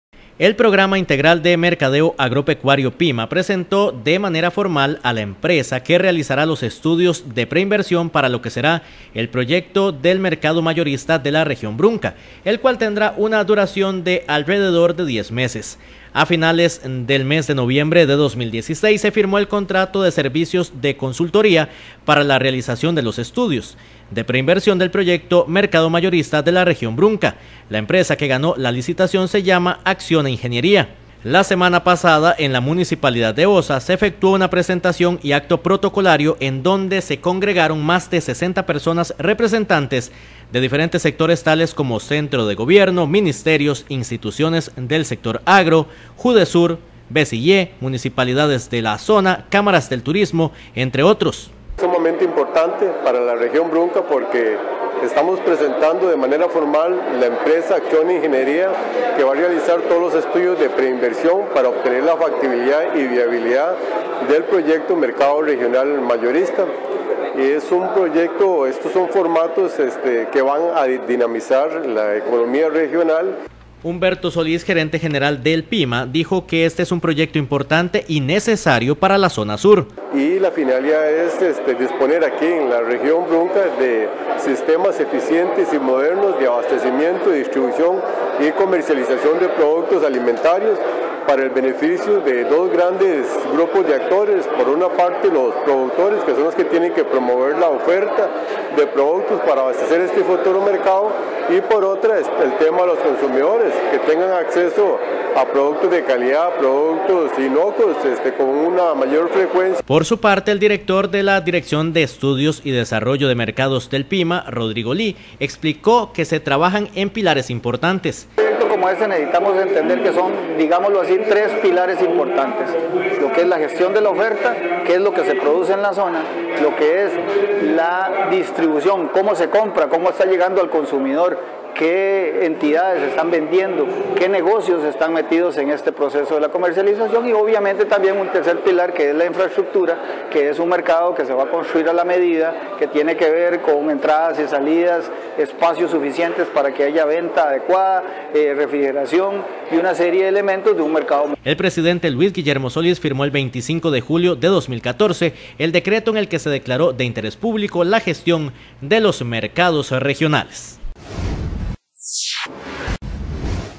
Presentan empresa que hará los estudios de pre-inversión del Mercado Regional Mayorista
El Programa Integral de Mercadeo Agropecuario PIMA, presentó de manera formal a la empresa que realizará los estudios de pre-inversión para lo que será el proyecto del Mercado Mayorista de la Región Brunca , el cual tendrá una duración de alrededor de 10 meses.